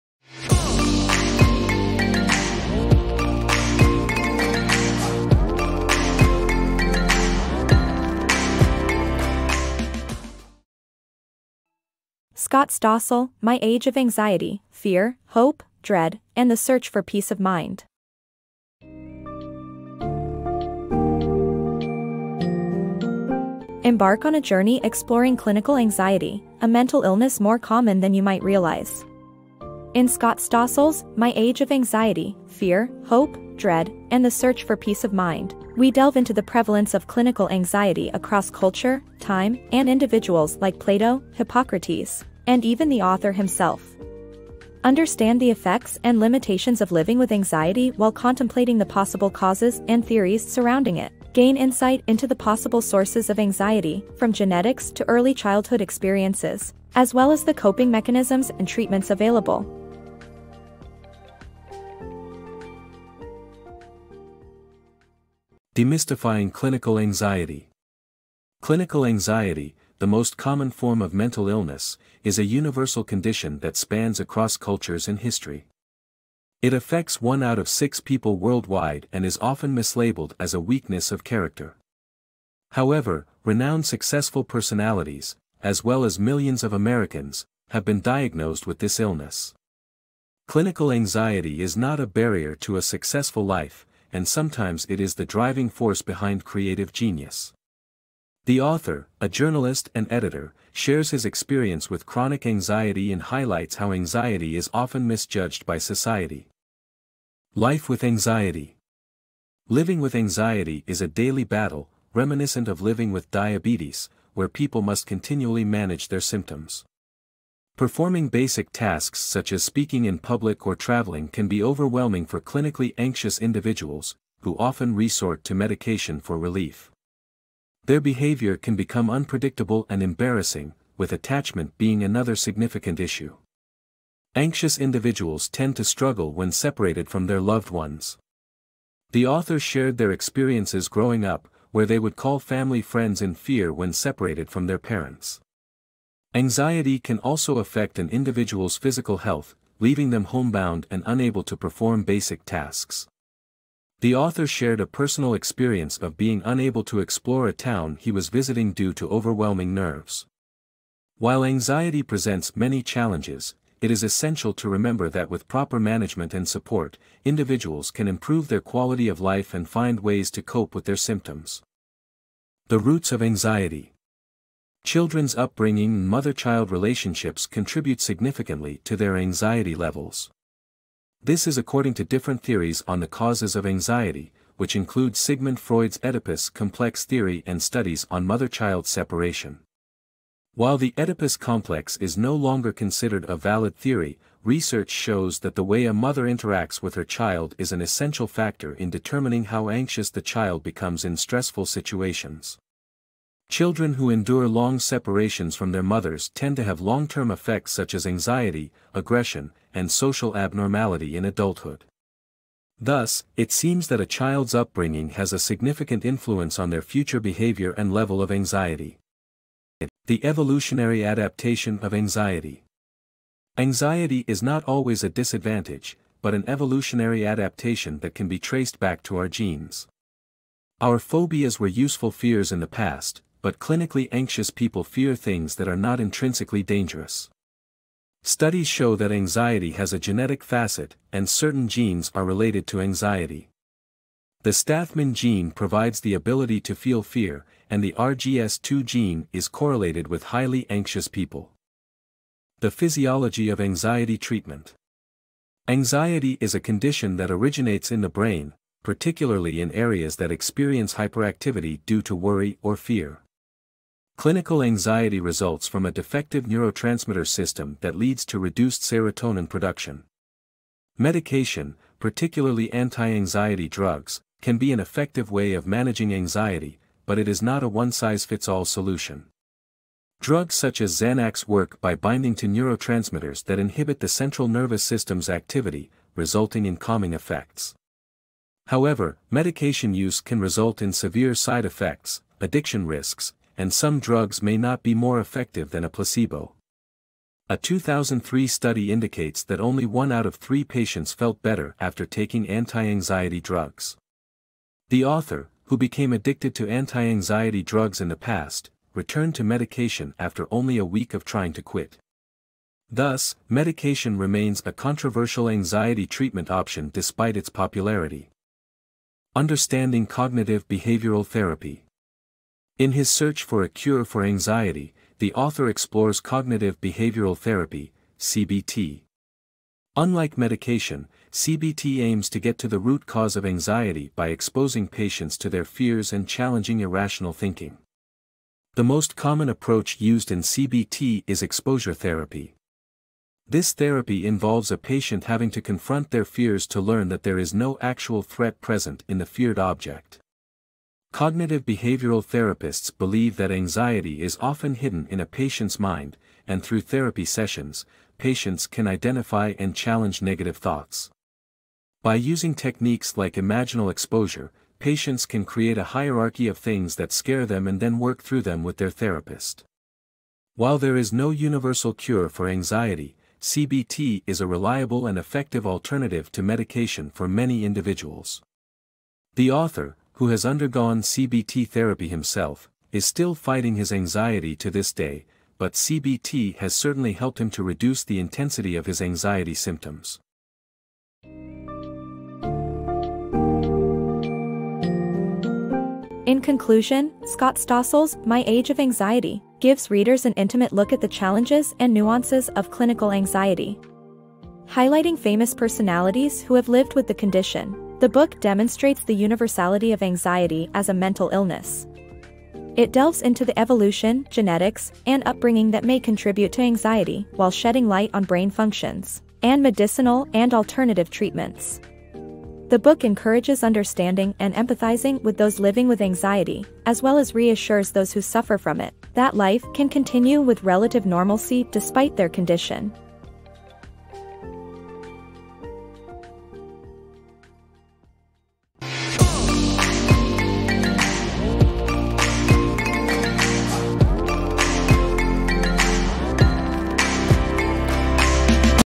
My Age of Anxiety by Scott Stossel: 7 Minute Summary
*BOOK SUMMARY*

TITLE - My Age of Anxiety: Fear, Hope, Dread, and the Search for Peace of Mind

AUTHOR - Scott Stossel

DESCRIPTION:
📖 In "My Age of Anxiety," Scott Stossel delves into a condition that affects millions worldwide. This captivating summary explores the science, history, and treatment of anxiety while sharing Stossel's harrowing personal journey. Gain insight and empathy with this learned and inspirational book.

TIMESTAMPS:
⌚ 00:00 Introduction
⌚ 01:06 Demystifying Clinical Anxiety
⌚ 01:48 Life with Anxiety
⌚ 02:53 The Roots of Anxiety
⌚ 03:40 The Evolutionary Adaptation of Anxiety
⌚ 04:13 The Physiology of Anxiety Treatment
⌚ 05:20 Understanding Cognitive Behavioral Therapy
⌚ 06:31 Final Recap